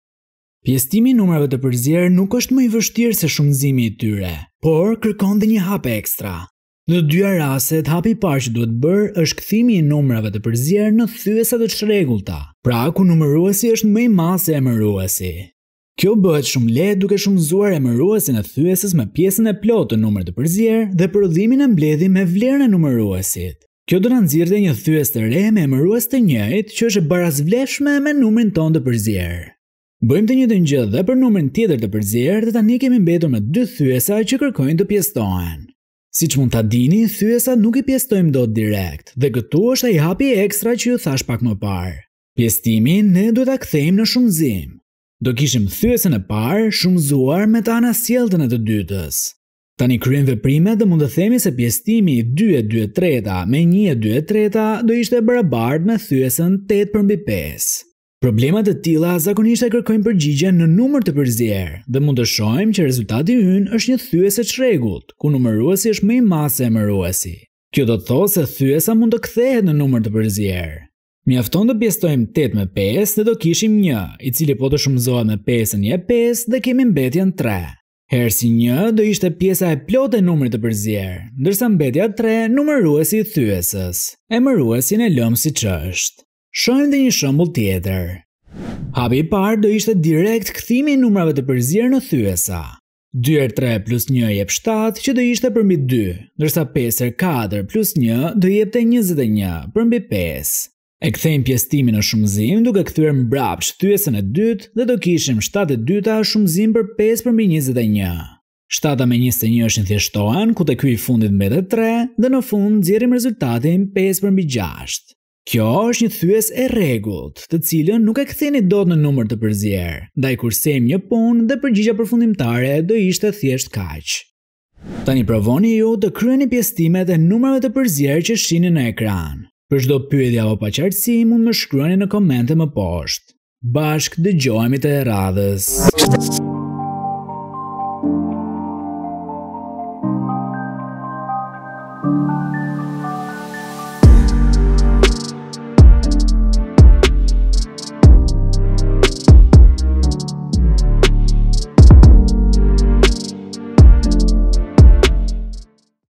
Pjesëtimi i Numrave të Përzier | Numrat e Përzier | Thyesat | Aritmetikë | Matematikë
Leksion ku do shohim si të gjejmë herësin e numrave të përzier.